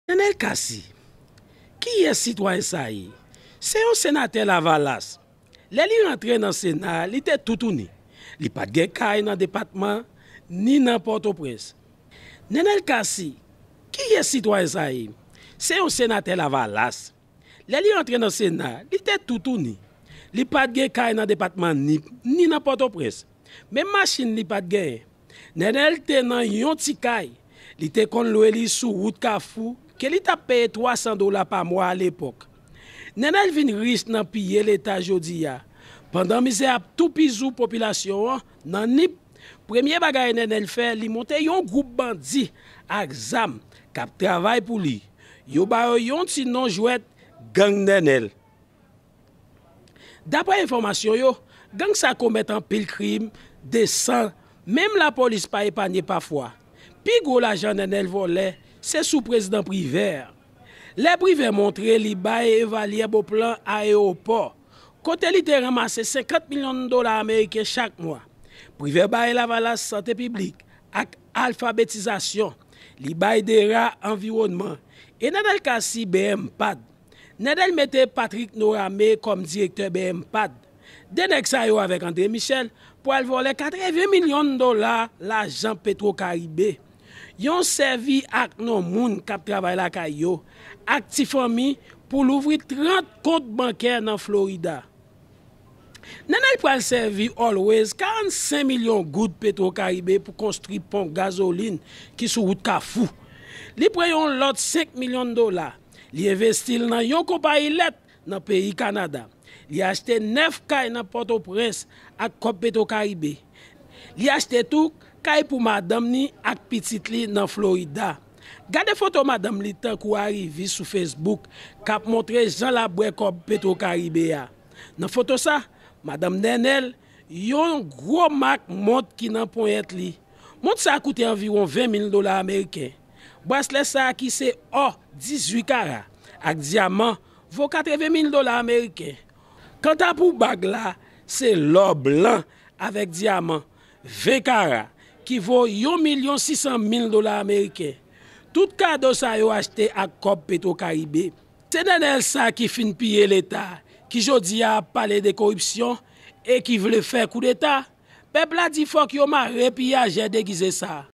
Detay sou kijan lavalas Nènèl Cassy vòlè kòb petrokaribe e vinn milyonè
Nènèl Cassy piye e vòlè leta ra pye tè. Li piye 80 milyon dola petrocaribe ak Patrick Norame et Andre Michel. Video sa eksplike detay yo.